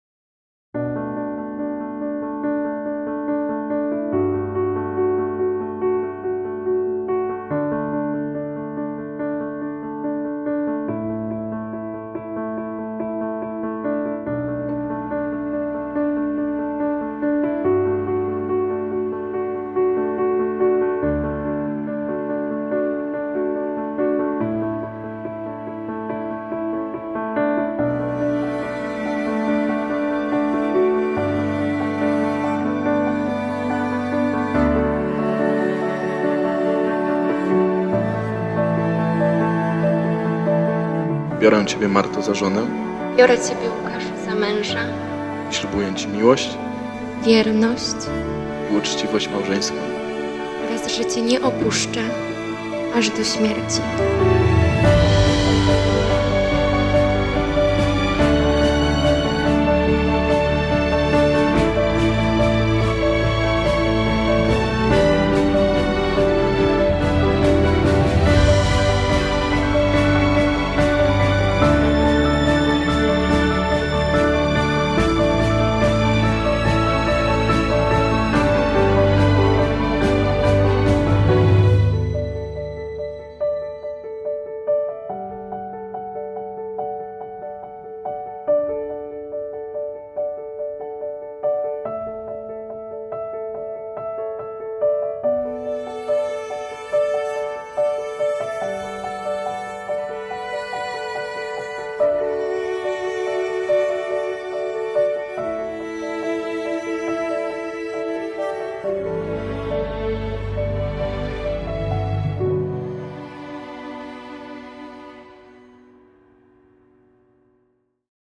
Trailer cudownego wesela w namiocie nad jeziorem | KarenMedia
Zapowiedź filmu ślubnego z pięknego, wielkiego wesela zorganizowanego pod namiotem . Teledysk ślubny